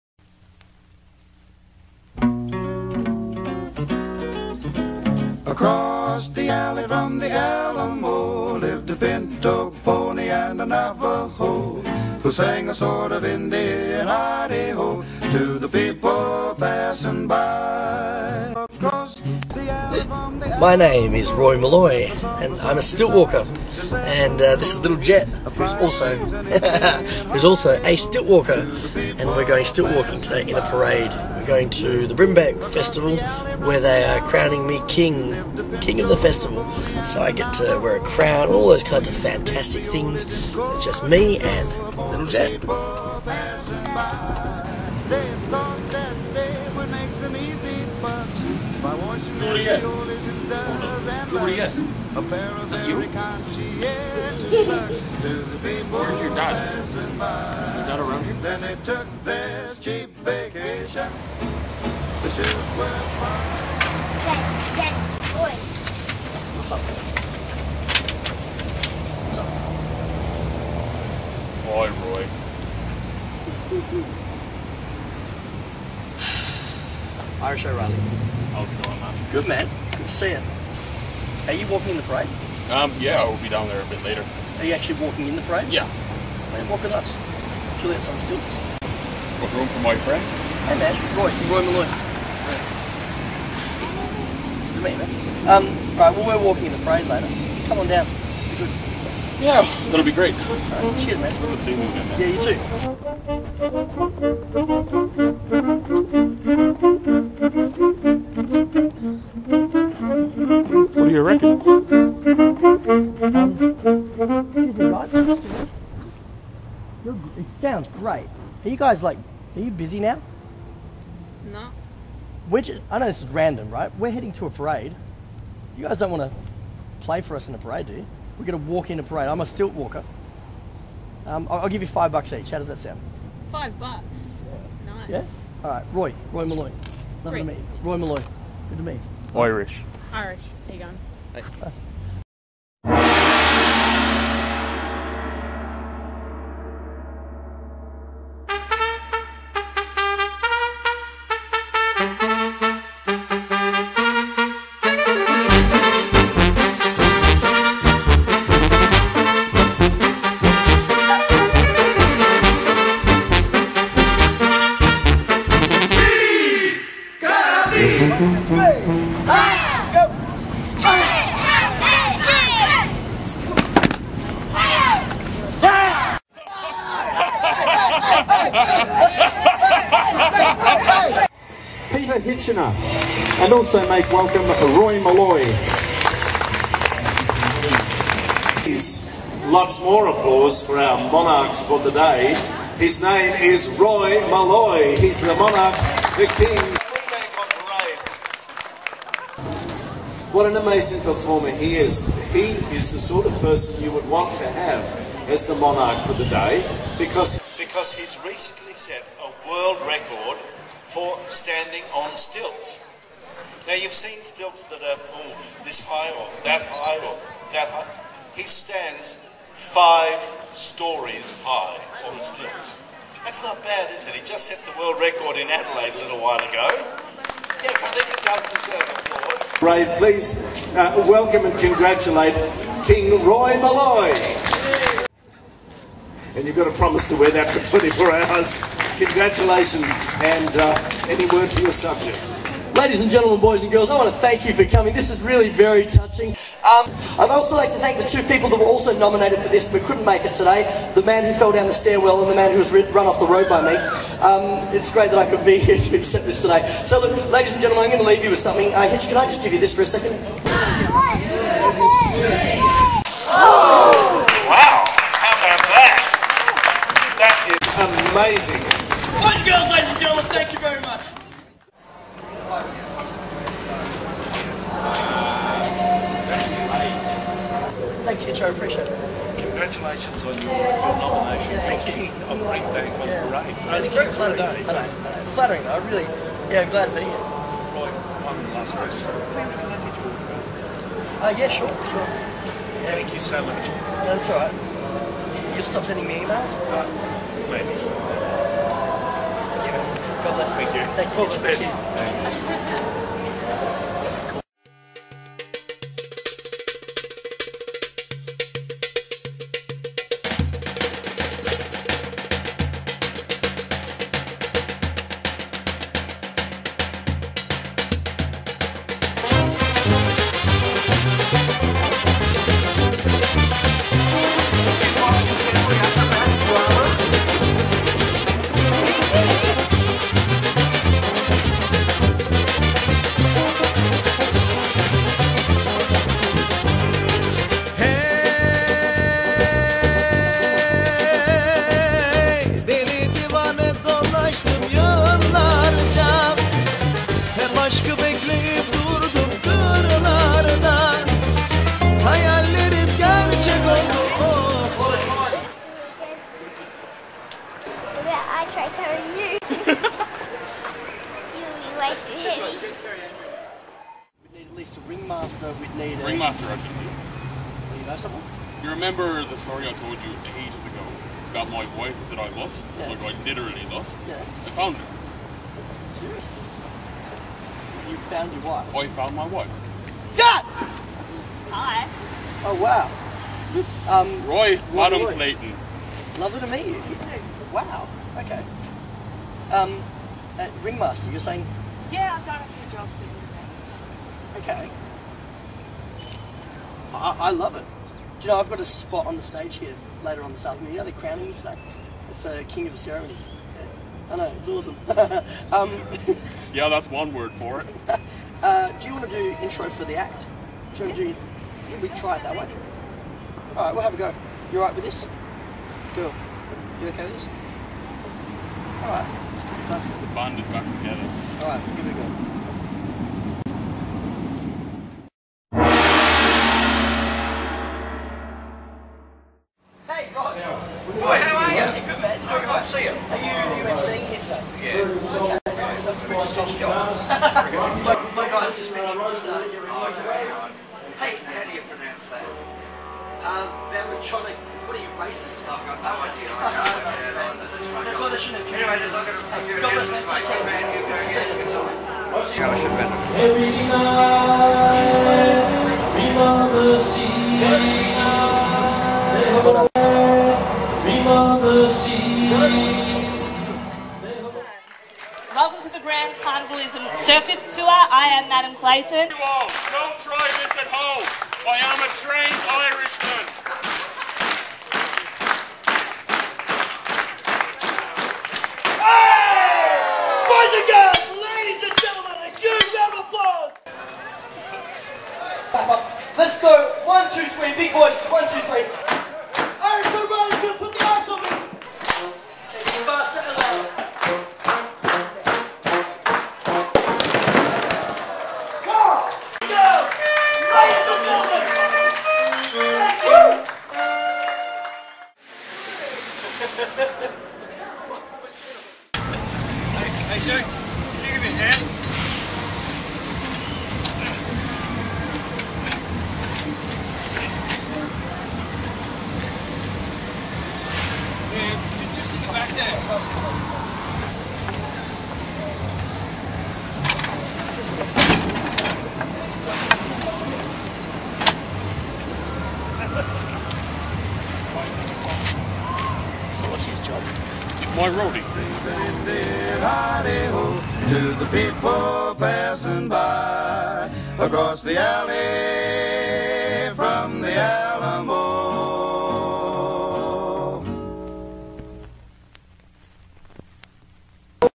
King of the Parade - Street Parade With Stilt Walker - Sunshine Brimbank
As Roy Maloy begins a new year and season he is crowned King of the Sunshine/Brimbank Festival when Irish O'Reilly finds him once more.